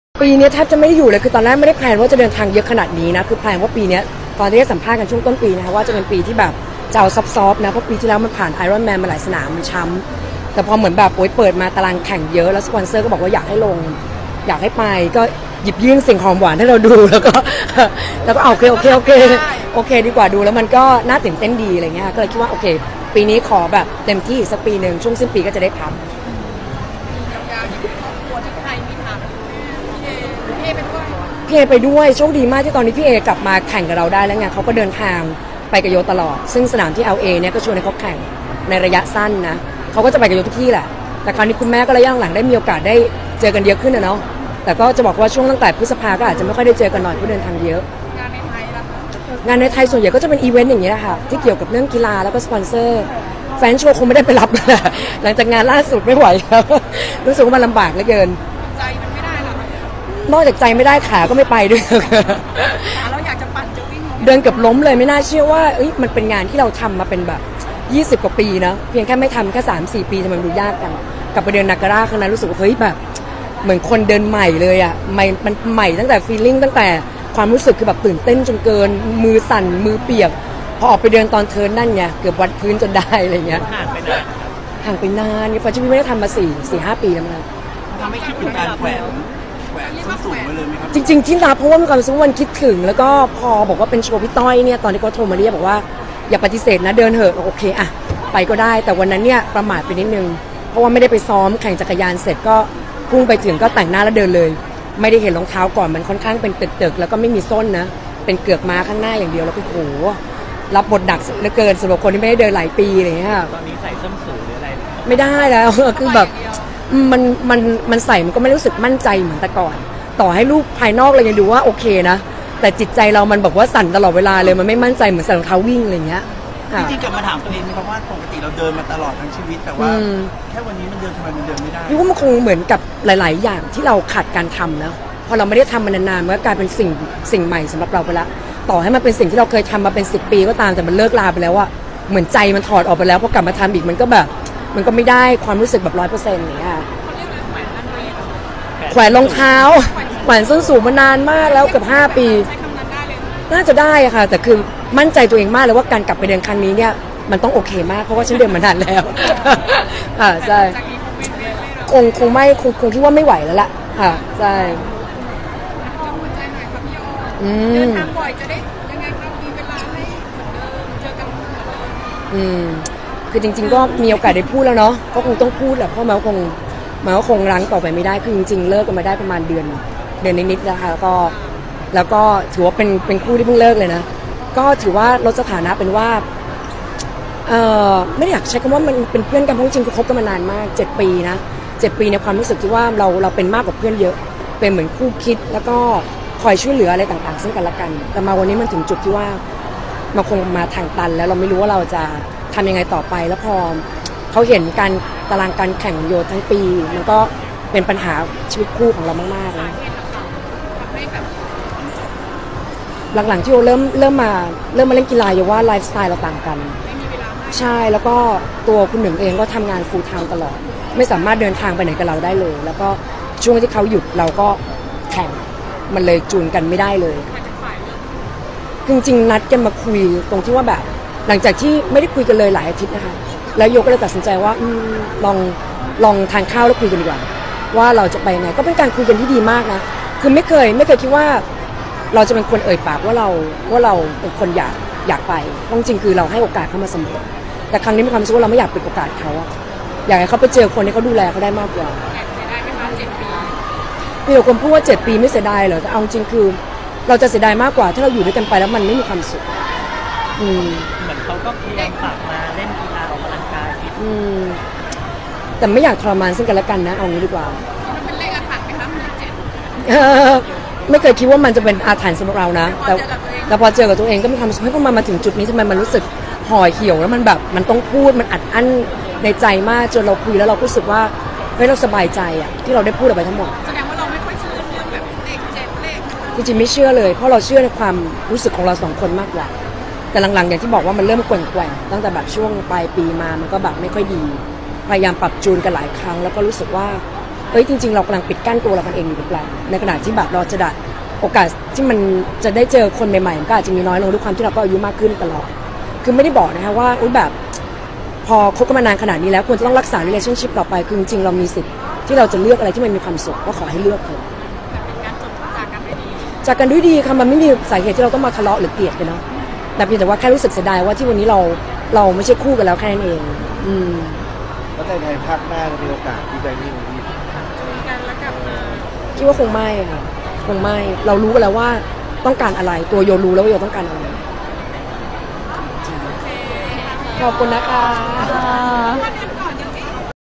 "โย ยศวดี" รัก 7 ปีถึงทางตัน ตัดใจยุติสัมพันธ์ "หนึ่ง" ไม่อยากรั้งให้ต่างฝ่ายต้องทรมาน
"โย ยศวดี" รัก 7 ปีถึงทางตัน ตัดใจยุติสัมพันธ์ "หนึ่ง" ไม่อยากรั้งให้ต่างฝ่ายต้องทรมาน 17/04/2019